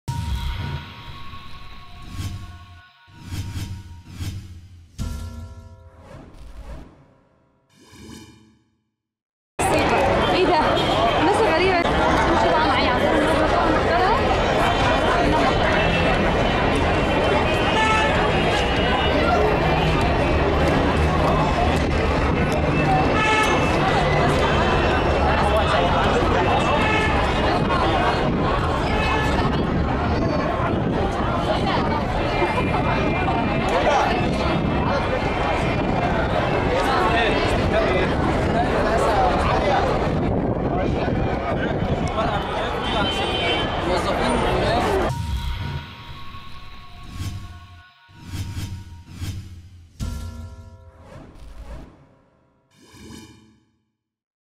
استفتاء انتخابات الرئاسة _جامعة القاهرة
استفتاء انتخابات الرئاسة كلية اقتصاد وعلوم سياسية القاهرة. شبكة يقين فى قلب الحدث